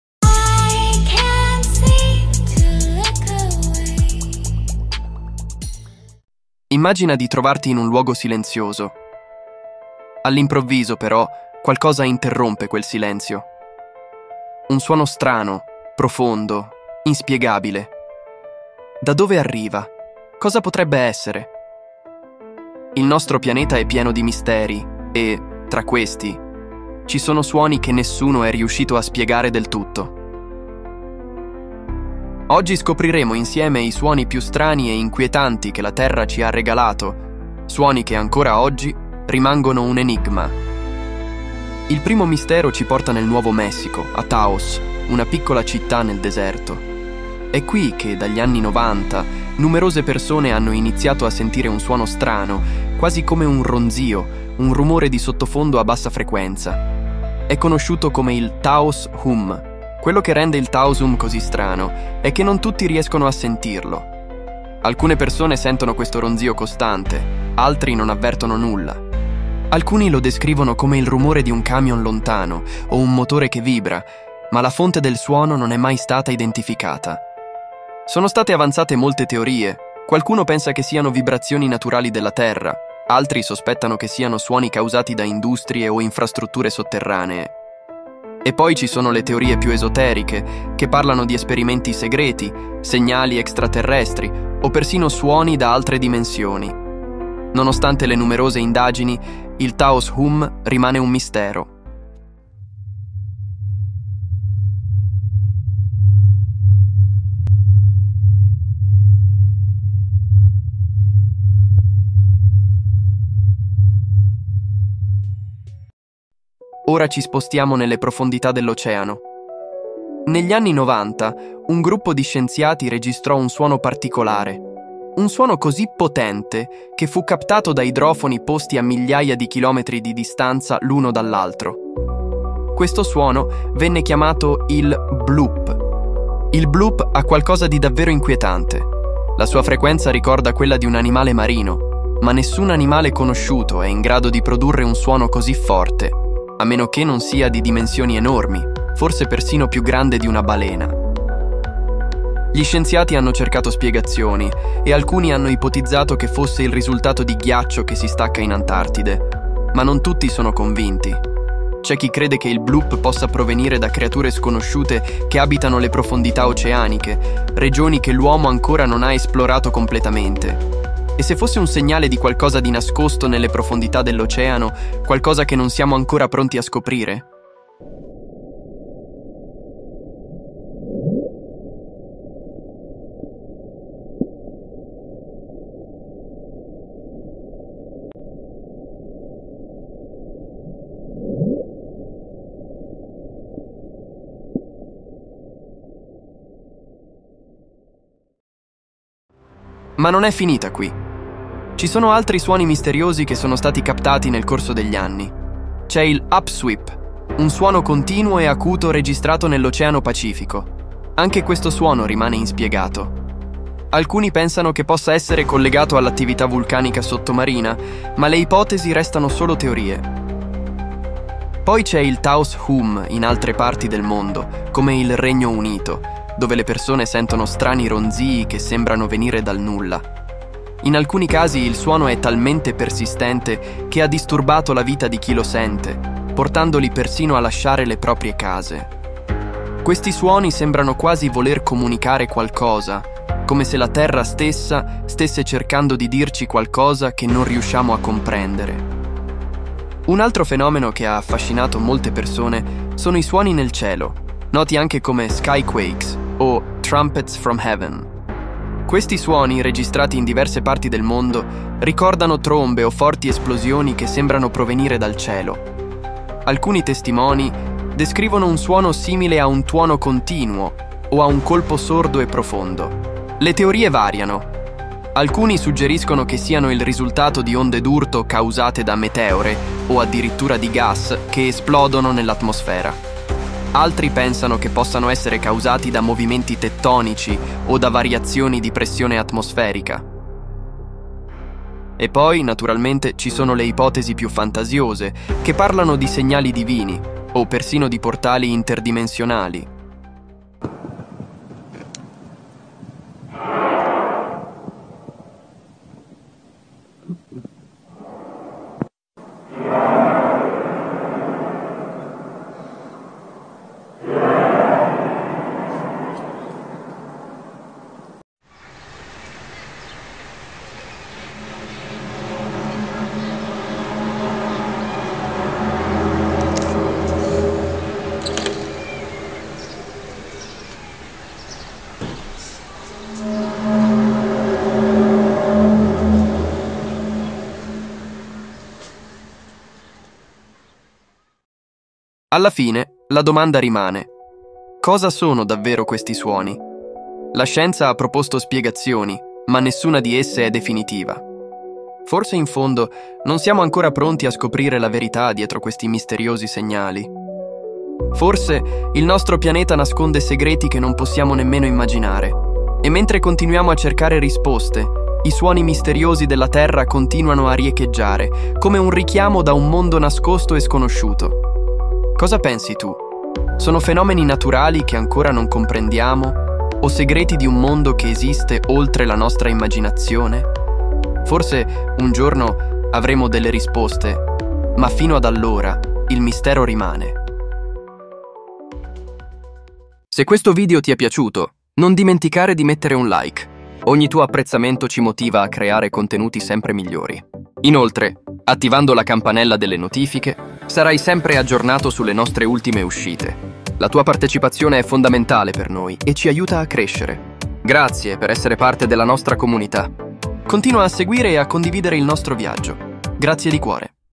Fenomeni Sonori: I Suoni più Enigmatici della Terra
In questo video esploriamo alcuni dei suoni più misteriosi della Terra, come il Taos Hum, il Bloop, e gli enigmatici suoni nel cielo. Sono fenomeni naturali ancora senza spiegazione, che affascinano scienziati e persone di tutto il mondo. Scopri con noi le ipotesi più interessanti e lasciati avvolgere dal mistero di questi segnali provenienti dalle profondità del nostro pianeta.